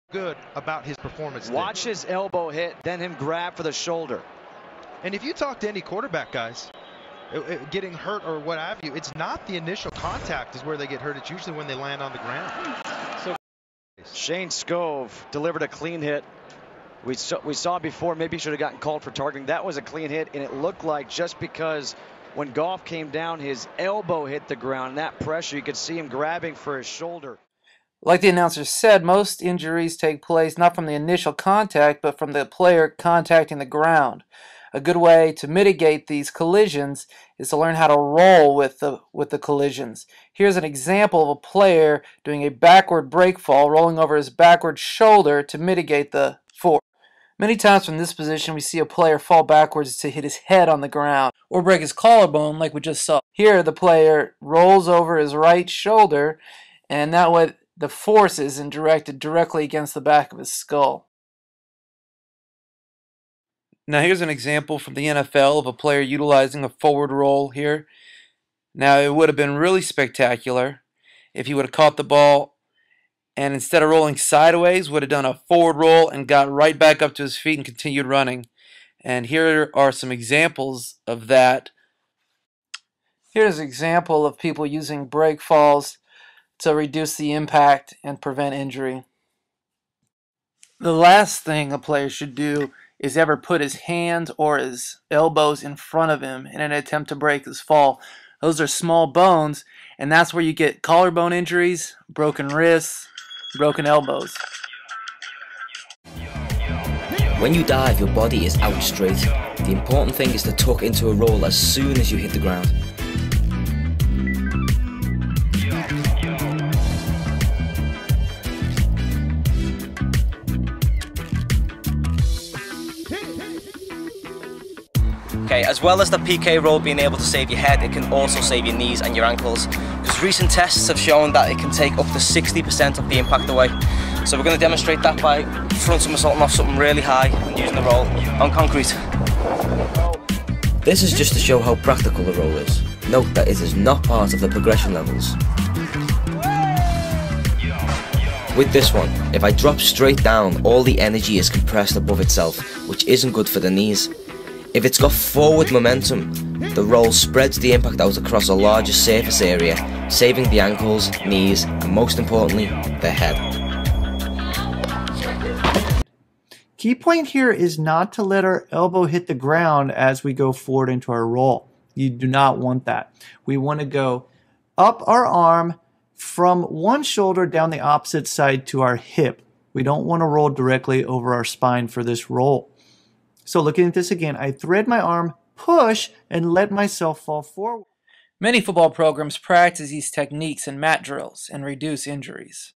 Football Concussion and Injury Prevention
Teaching athletes how to fall reduces injuries and improves level of play.